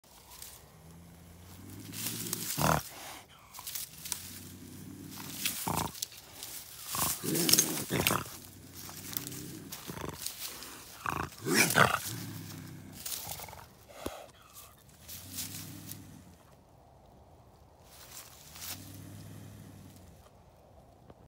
BIG Bobcat trapped legally! He’s not happy…
Bobcat not pleased in Georgia!